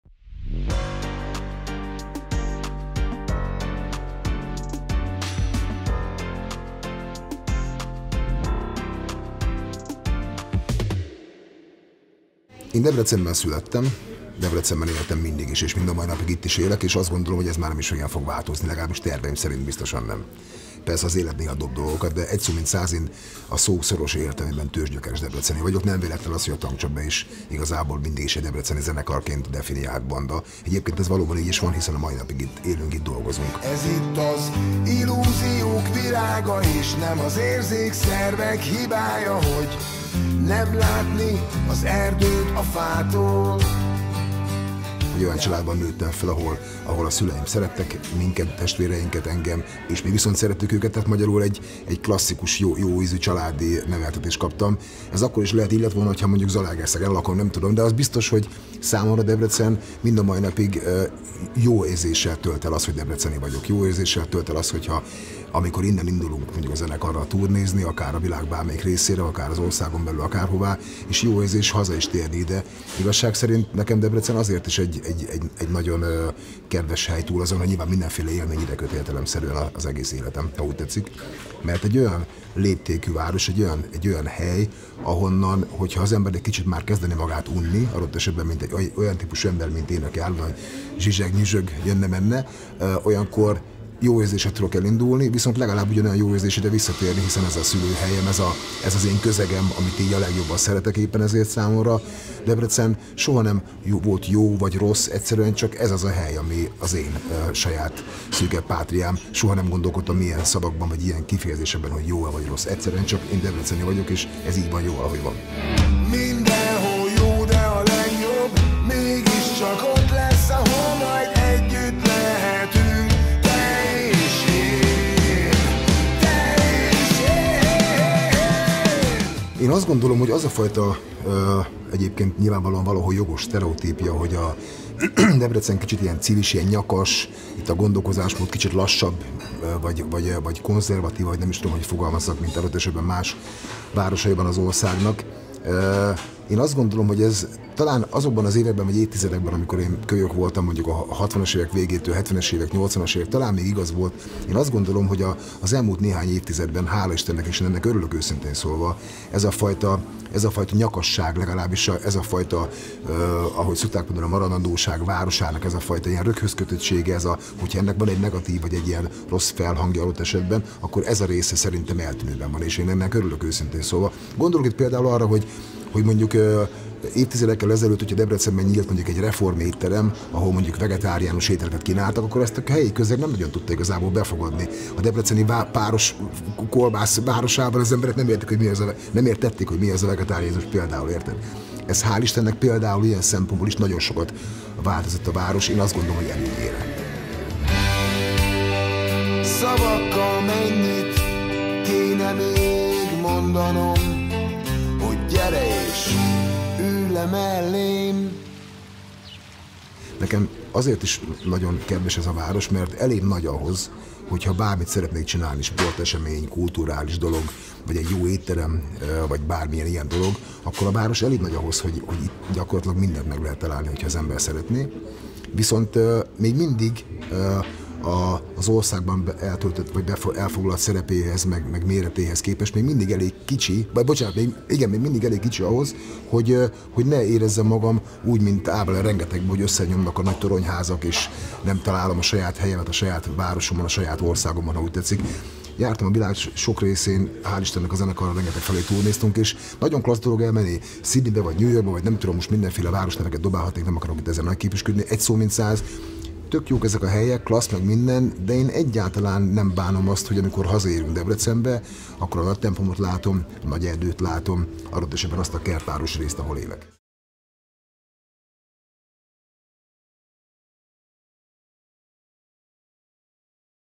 Lukács László, a Tankcsapda együttes alapítója és énekese
*Scroll down to English (and turn on the subtitle)!*

Ki a cívis? Milyen a cívisváros? Mit gondolnak a cívisségről és a debreceniségről azok az emberek, akik Debrecenben, Debrecenért élnek, dolgoznak, alkotnak nap mint nap?

A Déri Múzeum Cívisek világa címmel - a Magyar Géniusz Program jóvoltából - megújuló helytörténeti kiállítását előre vetítő sorozat a tárlat megnyitásáig hetente kétszer jelentkezik majd.

Who is a cívis? What is a cívis town like? What do the people who live, work and create in Debrecen, for Debrecen, every day, think about Debrecenity and about being a cívis?

A series previewing the Déri Museum's renewed local history exhibition entitled The World of the Cívis will be shown twice a week until the opening of the exhibition.